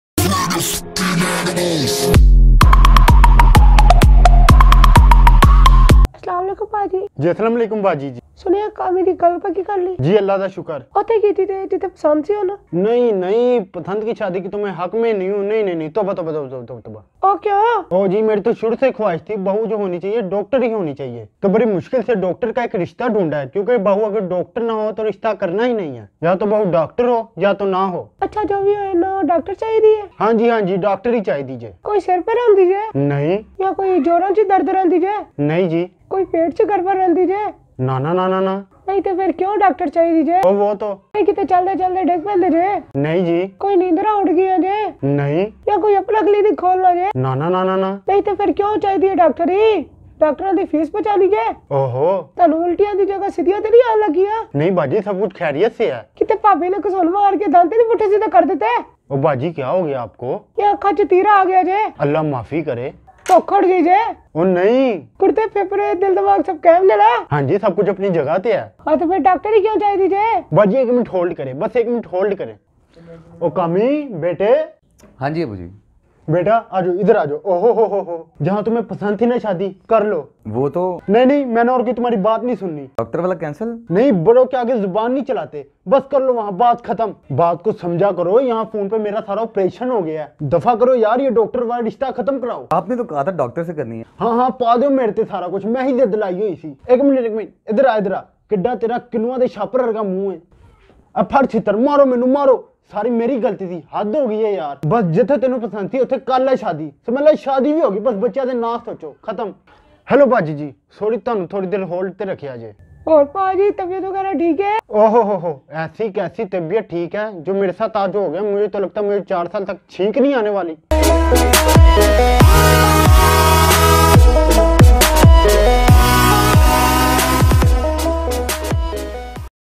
Pakistaniat Vines - Doctor Bahu
A Father Wants a Doctor Bahu.